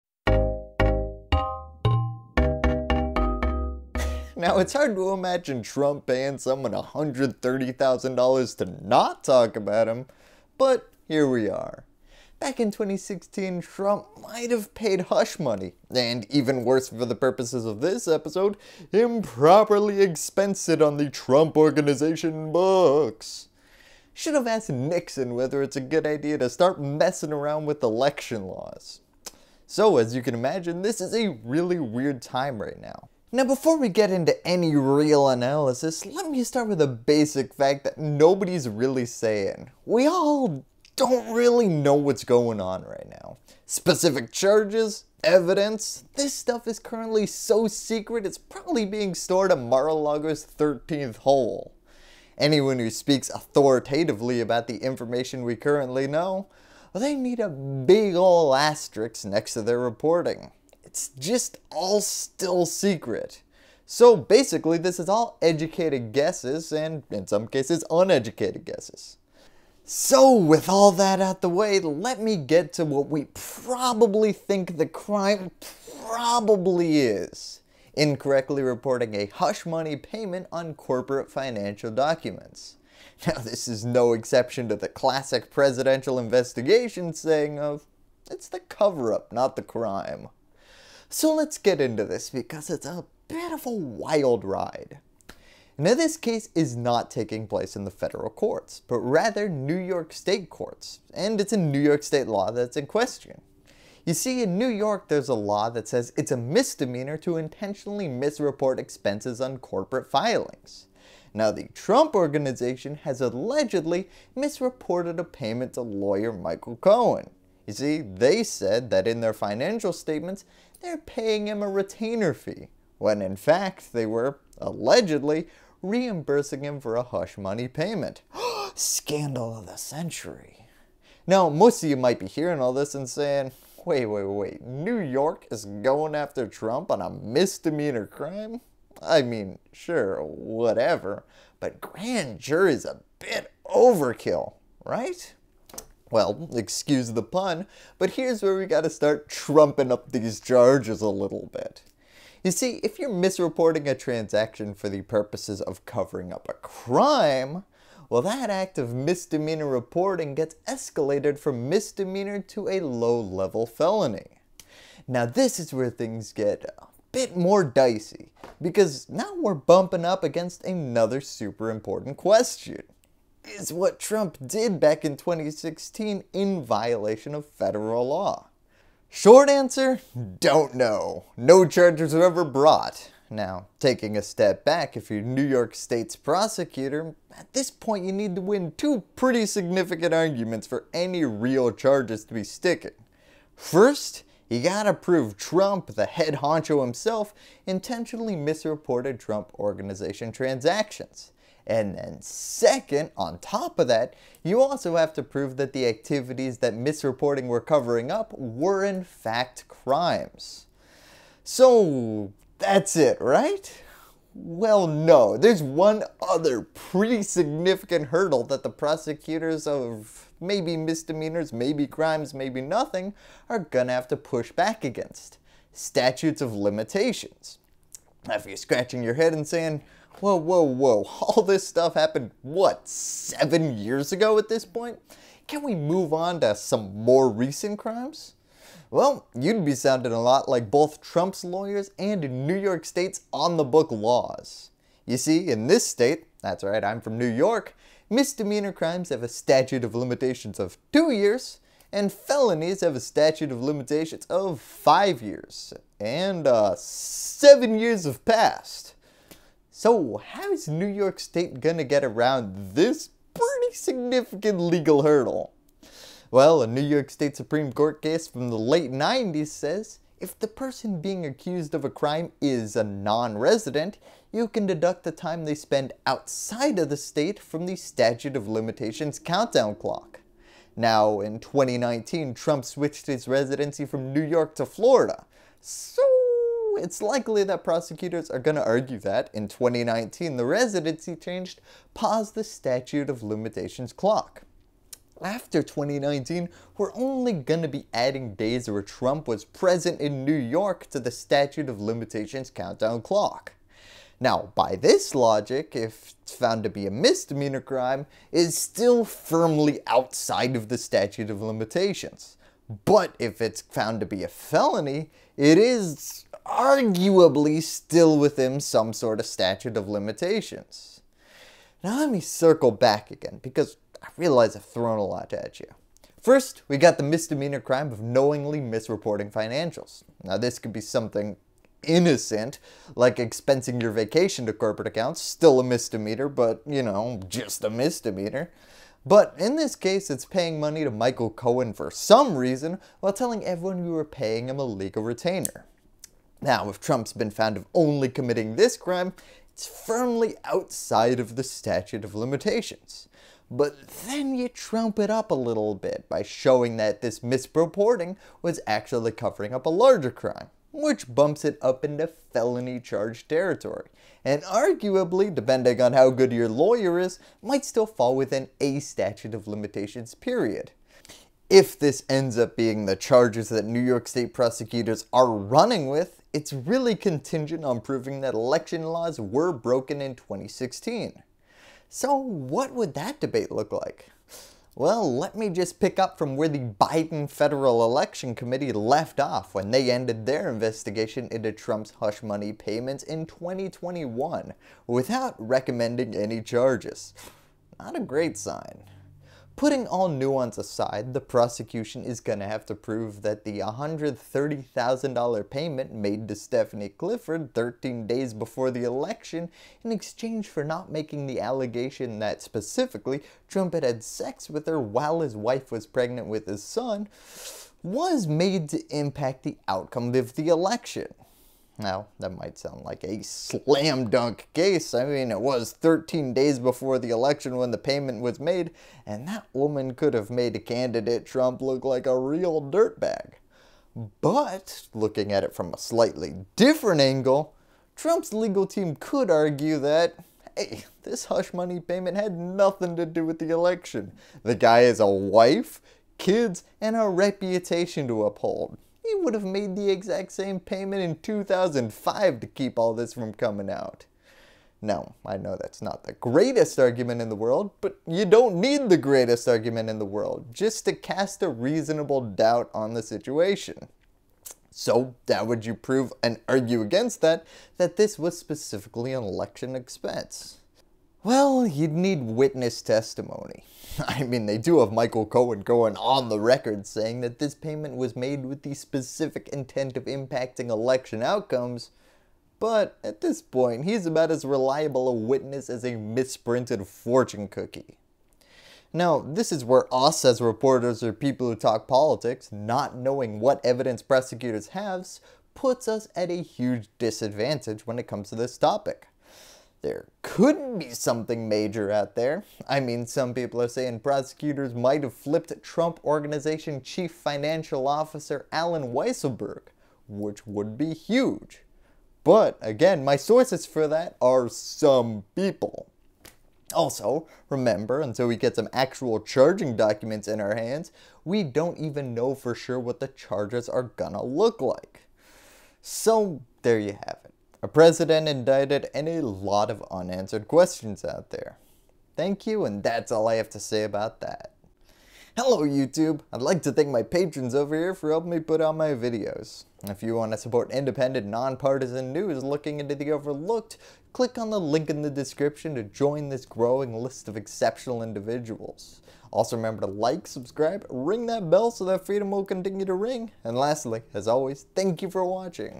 Trump Indictment: The Hush Money/Financial Fraud Charges, Explained
In 2016, Donald Trump allegedly paid hush money to Stephanie Clifford, AKA Stormy Daniels, and improperly expensed it on Trump Organization books. This led to a New York State investigation into whether Trump intentionally misreported financial transactions, which could escalate from a misdemeanor to a low-level felony if it was found to cover up a crime. However, with statutes of limitations of 2 years for misdemeanors and 5 years for felonies, the question remains: can Trump be prosecuted for a crime that allegedly took place over 7 years ago? This video delves into the legal hurdles facing the former president, including how New York State is attempting to get around the statute of limitations, and what would need to be proven for charges to stick.